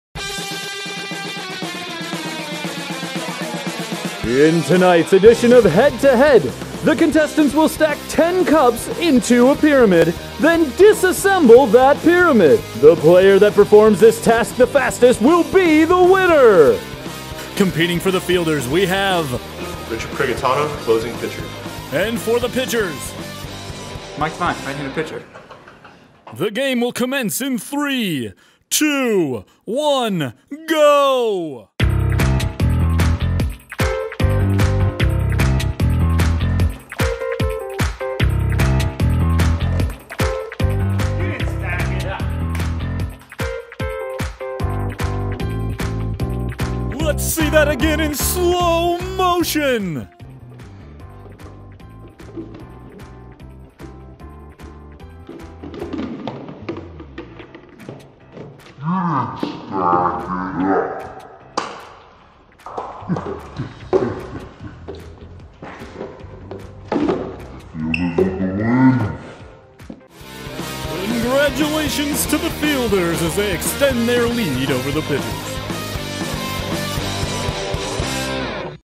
Head To Head | Cup Stack - Prigatano vs. Devine
Richard Prigatano and Mike Devine face off in a new Budweiser Head To Head challenge...Cup Stack